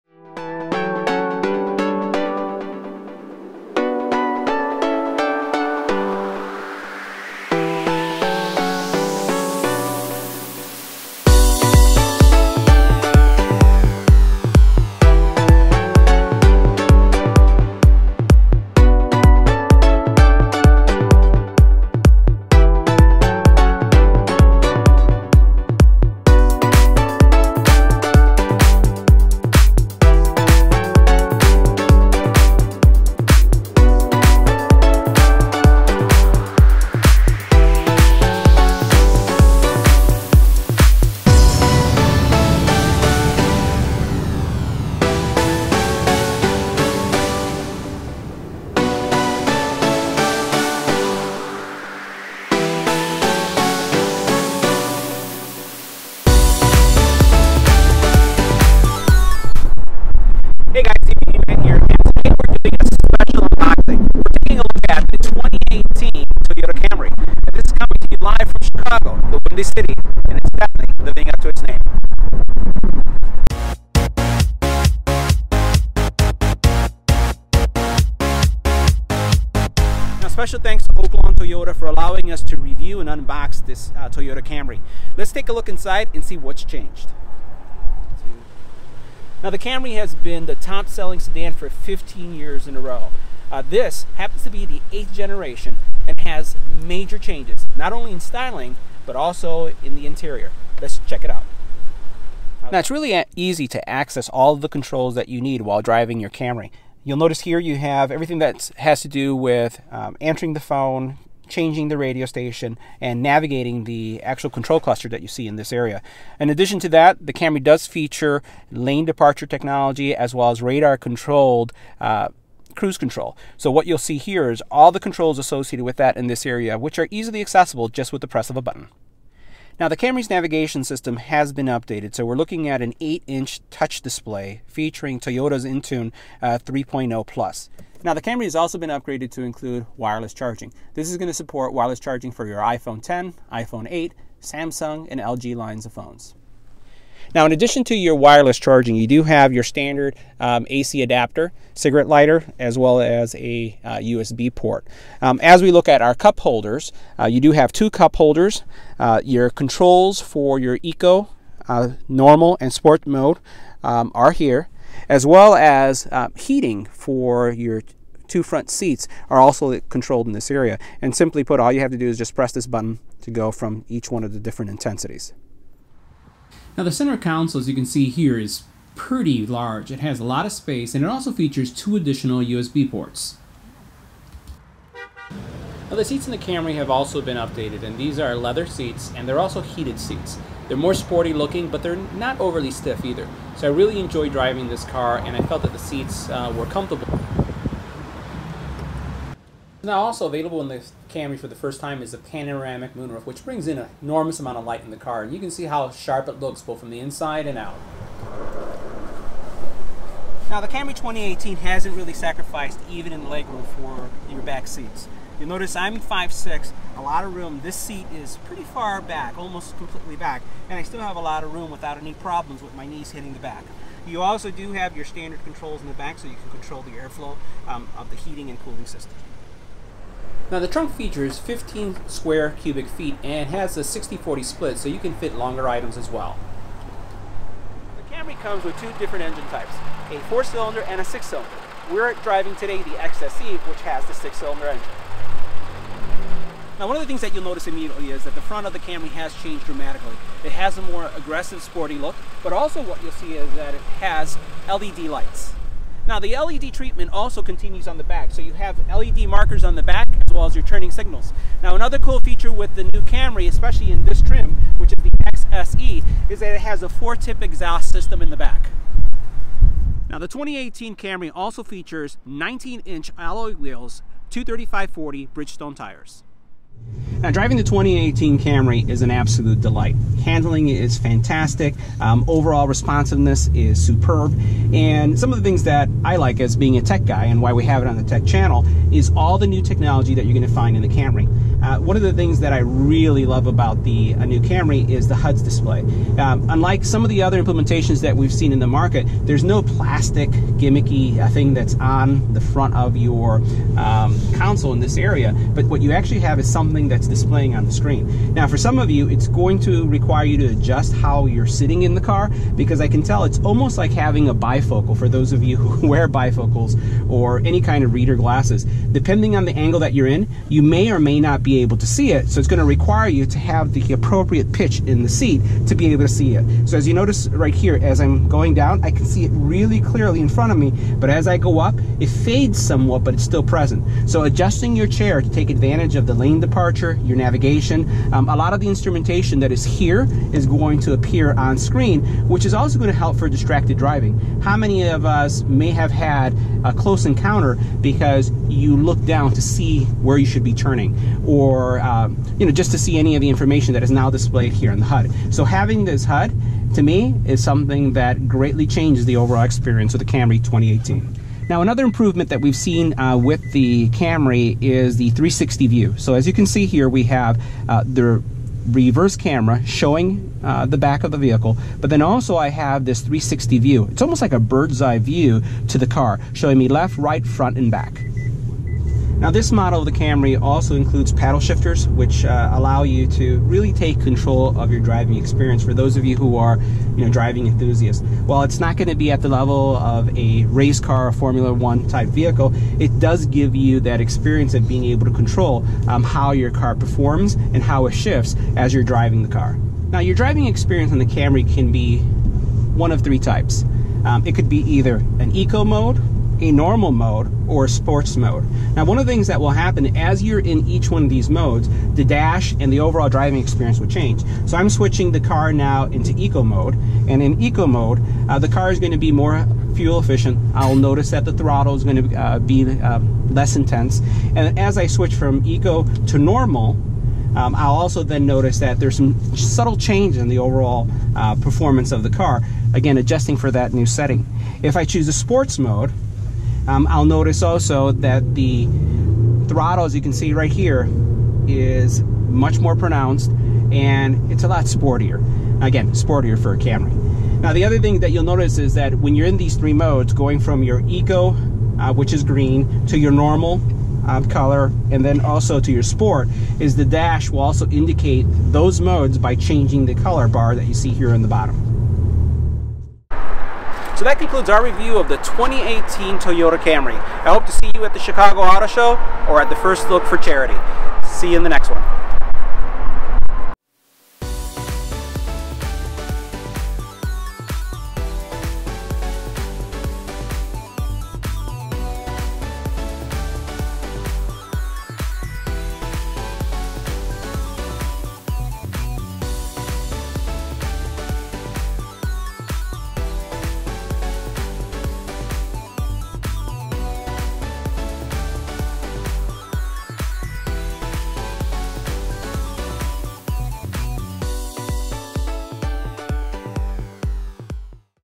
First Look | Toyota Camry 2018 XSE Unboxing Review and Test Drive
A few weeks ago I put a survey out on the community about covering the first look for charity auto show event.  Over 78 Percent of you said you would like it covered. So here is are first review.  This is the 2018 Toyota Camry.   It was WINDY in chicago Very WINDY so audio at first took a beating,  Many thanks to Oak Lawn Toyota for allowing us to review the Toyota Camry 2018  XSE

- ebpman